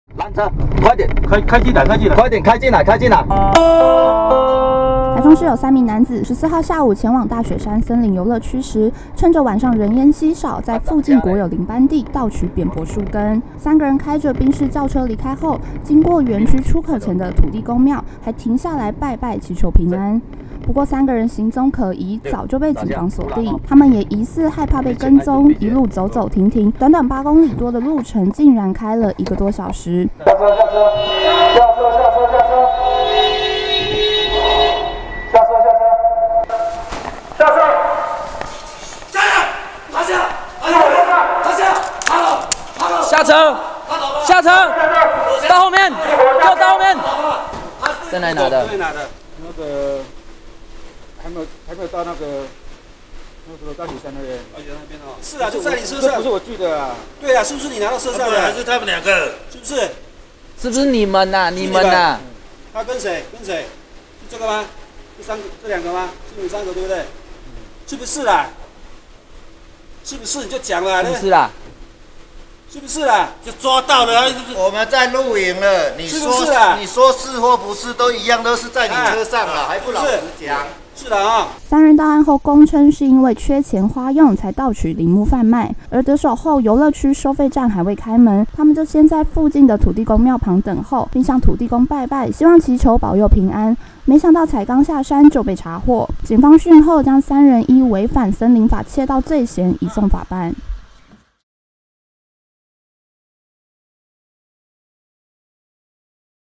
囂張山老鼠 大雪山竊扁柏還求土地公保佑(保七總隊第五大隊提供)
相關新聞:

囂張山老鼠 大雪山竊扁柏還求土地公保佑

3男「車床」大雪山森林遊樂區盜伐 拜完土地公後被逮

台中市43歲廖姓男子、42歲吳姓男子與48歲楊姓男子，14日下午前往大雪山森林遊樂區遊憩時，趁著晚上人煙稀少，竟在今天凌晨前往附近國有林班地獲盜伐的扁柏樹根材4塊、扁柏元寶半成品，之後廖男等人開著賓士轎車要離開時，竟還到園區出口前的土地公命拜拜求平安，不過廖男等人因形跡可疑早被鎖定，警方一舉逮人，今天訊後違反森林法竊盜罪嫌移送。